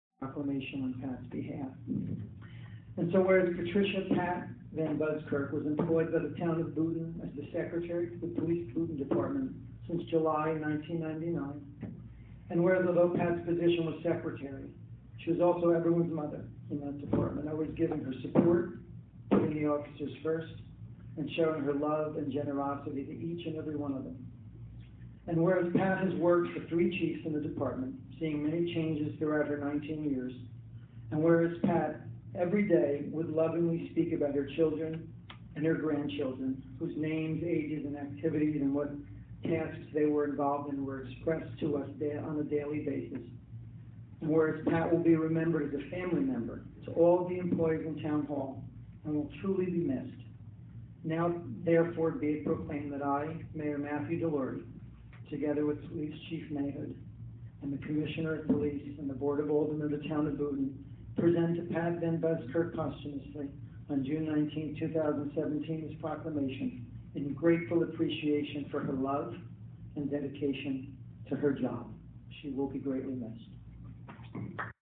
Boonton plaque
Boonton Mayor Matt DiLauri presents a plaque of appreciation on behalf of the town to the family of Patricia VanBuskirk. VanBuskirk was the long-time secretary to the police department. The plaque was presented at the Boonton Board of Aldermen meeting on Monday, June 19.